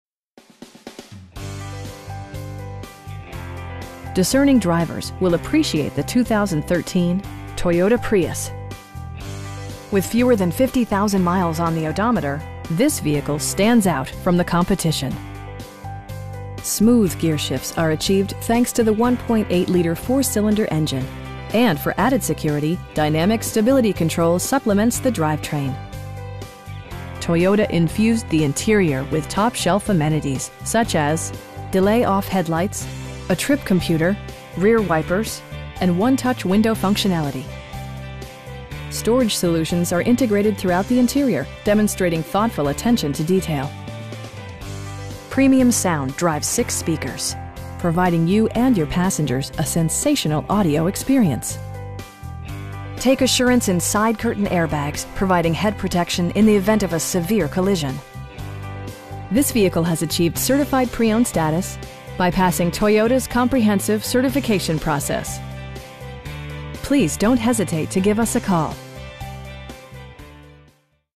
2013 Toyota Prius TWO in Vienna, VA 22182
Discerning drivers will appreciate the 2013 Toyota Prius. With fewer than 50,000 miles on the odometer, this vehicle stands out from the competition! Smooth gearshifts are achieved thanks to the 1.8 liter 4 cylinder engine, and for added security, dynamic Stability Control supplements the drivetrain. Toyota infused the interior with top shelf amenities, such as: delay-off headlights, a trip computer, front bucket seats, tilt steering wheel, an overhead console, rear wipers, and 1-touch window functionality. Storage solutions are integrated throughout the interior, demonstrating thoughtful attention to detail. Premium sound drives 6 speakers, providing you and your passengers a sensational audio experience. Take assurance in side curtain airbags, providing head protection in the event of a severe collision. This vehicle has achieved Certified Pre-Owned status, by passing Toyota's comprehensive certification process. You will have a pleasant shopping experience that is fun, informative, and never high pressured. Please don't hesitate to give us a call.